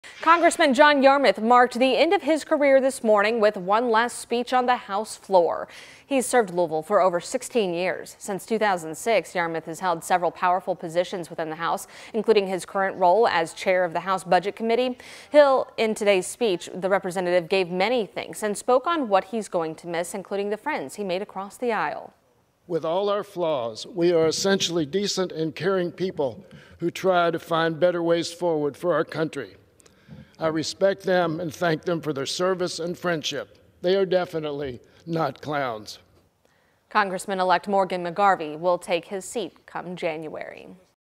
Kentucky Congressman John Yarmuth delivers last address in D.C.
Since 2006, Yarmuth has held several powerful positions within the house, including his current position as chair of the house budget committee.